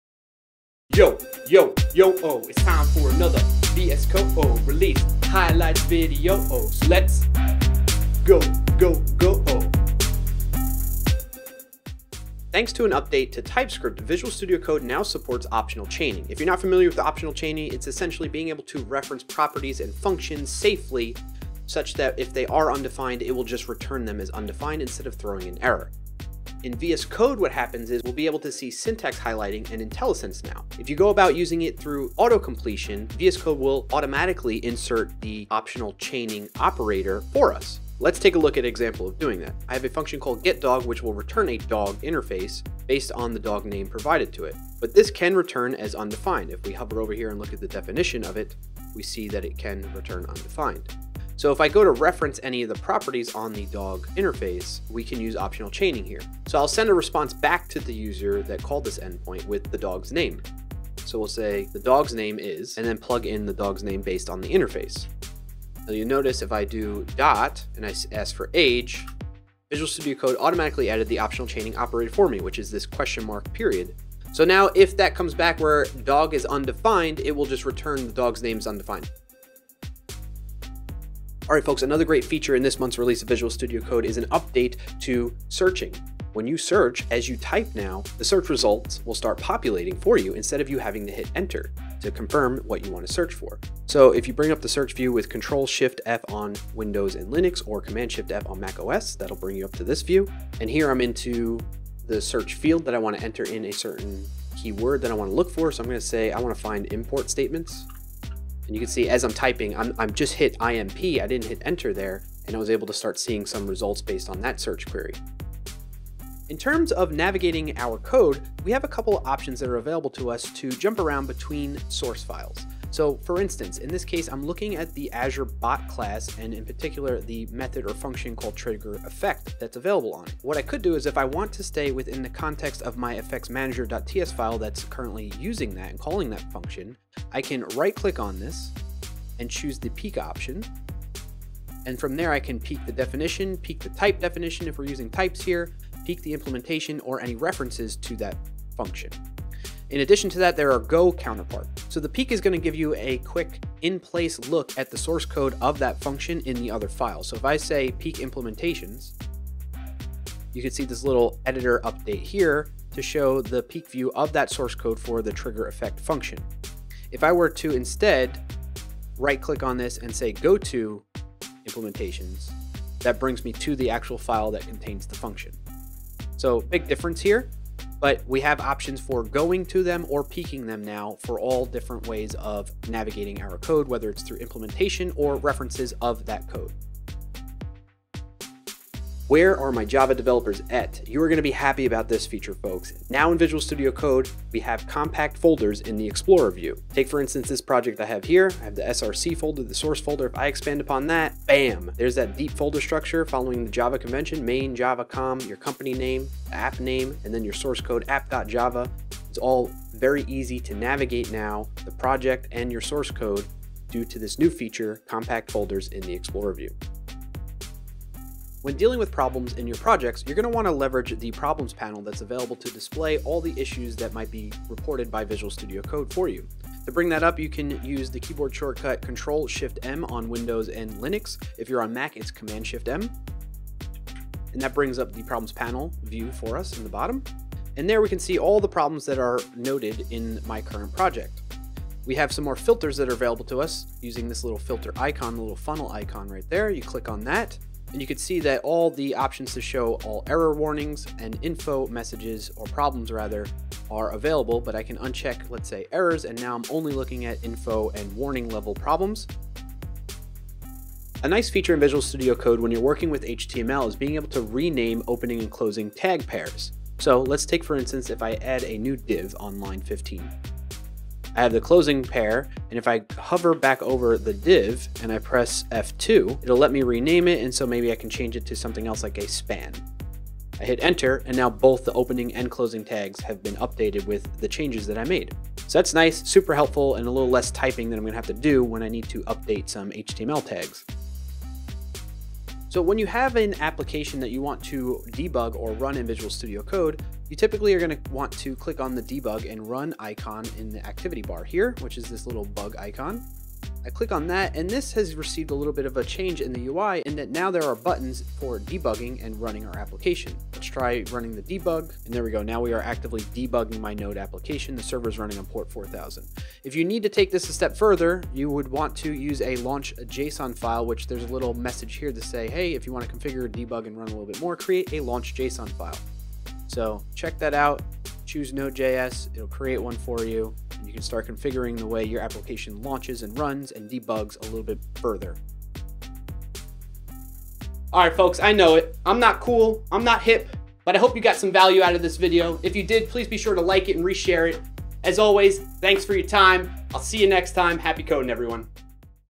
Visual Studio Code Release Highlights - November 2019
Highlights of some of the latest features found in the November 2019 release of Visual Studio Code.

Editing by Dhanish Gajjar